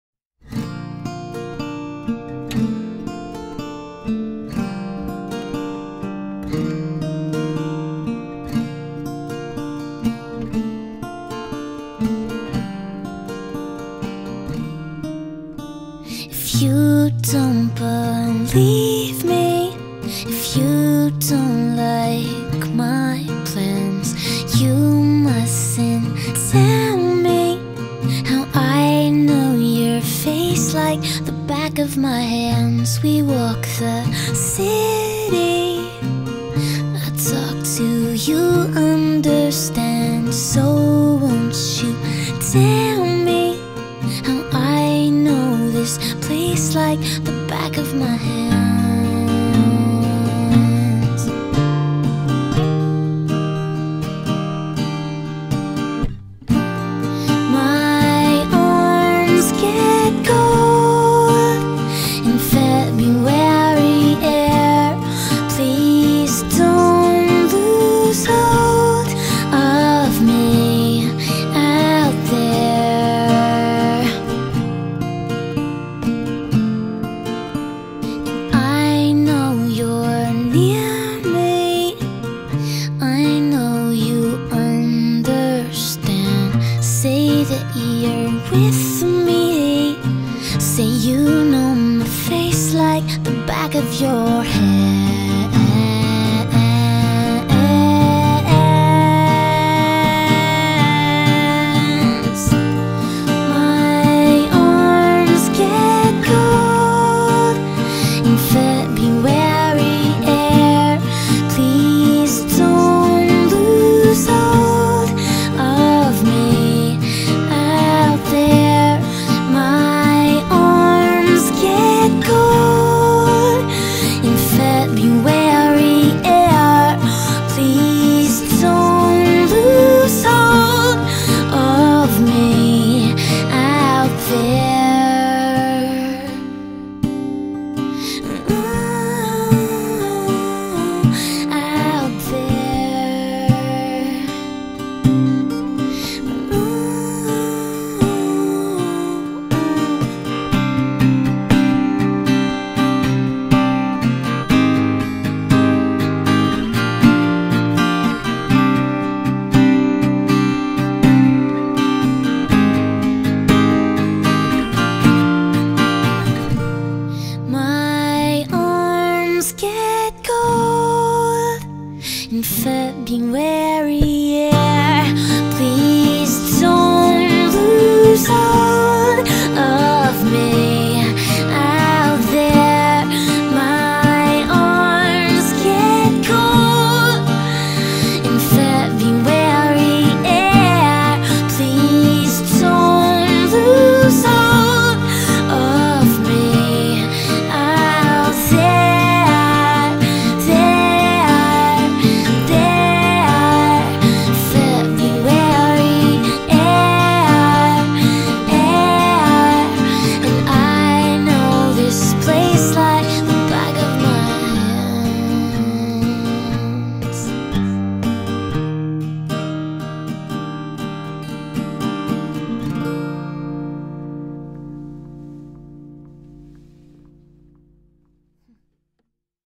Speed Edit | February Air ❄
Quite honestly just a crap ton of snowflakes. But, first concept/speed edit of 2015. Was originally going to keep a square crop but was convinced I liked the cropped version better. :)